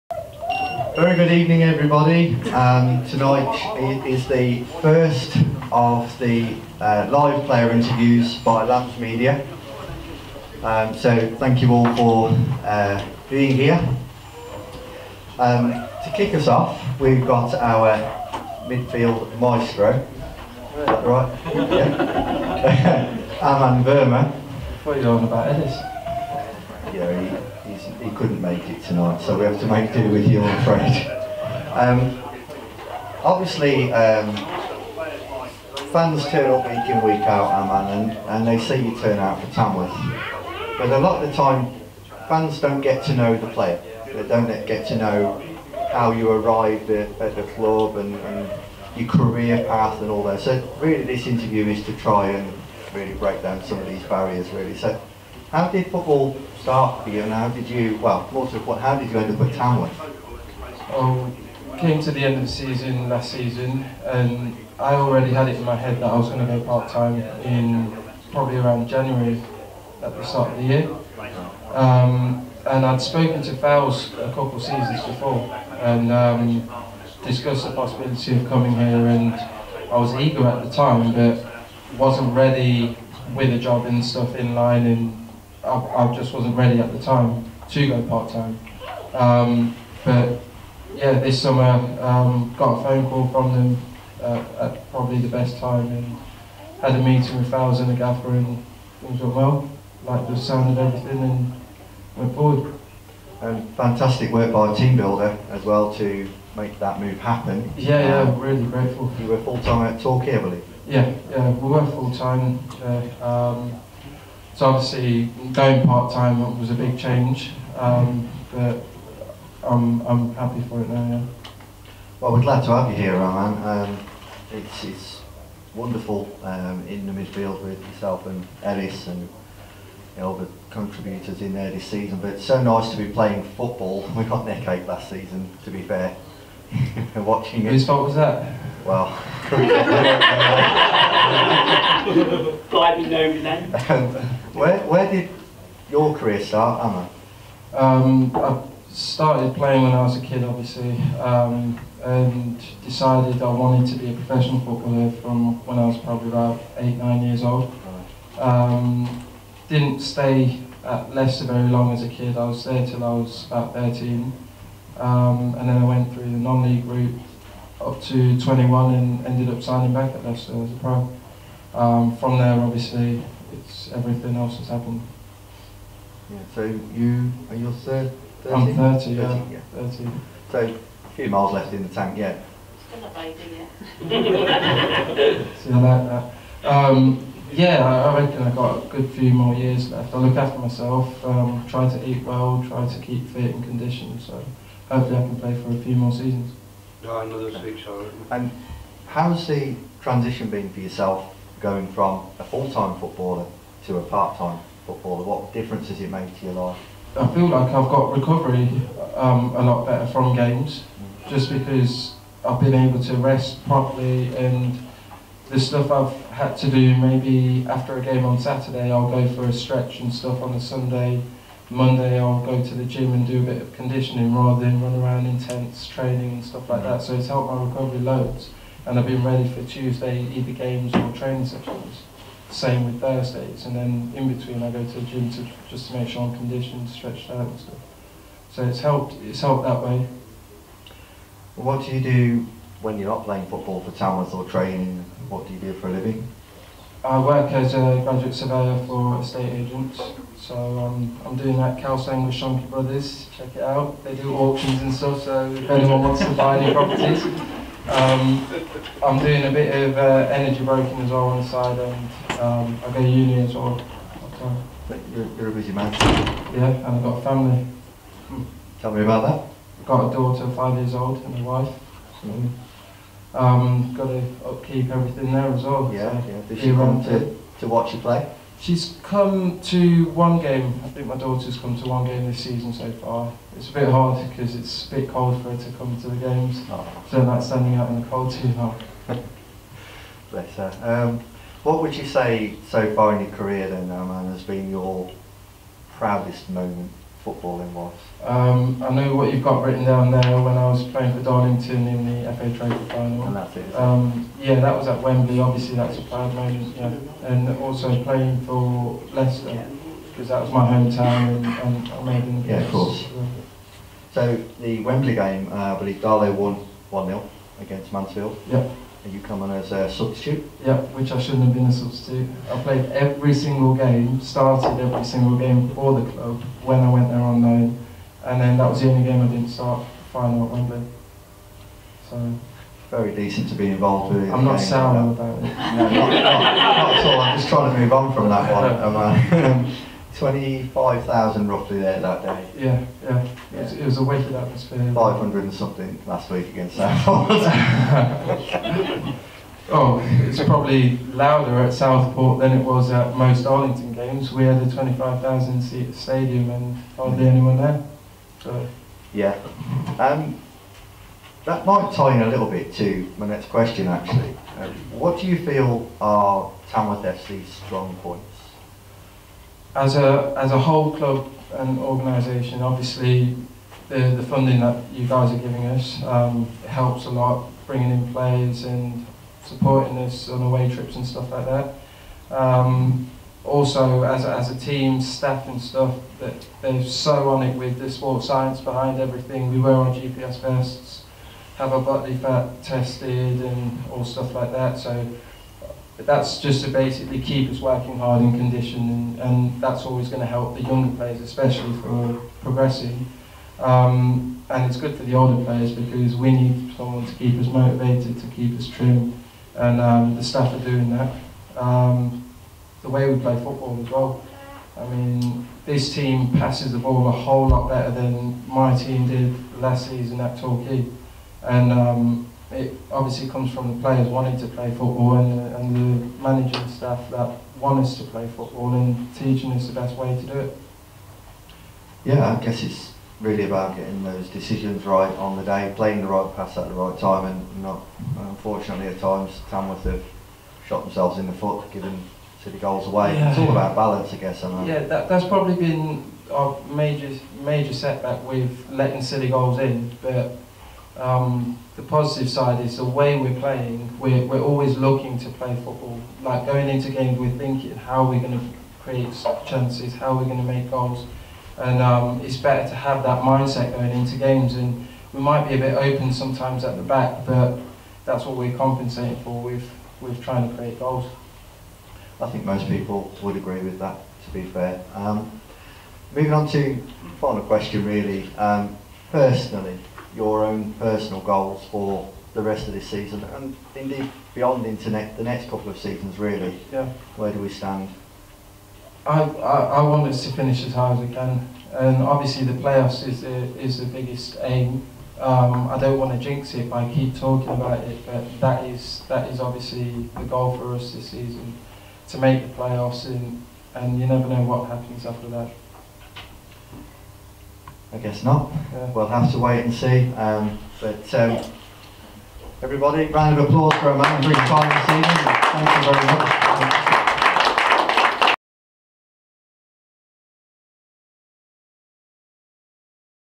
Aman Verma Live Player Interview
A Lambs Media First Live interview with Tamworth Midfielder Aman Verma. Filmed on the 14th December By Harry Hanslow with Questions from Kev Clarke. The next Live Interviews are with Callum Powell and Zack Kotwica on the 11th January!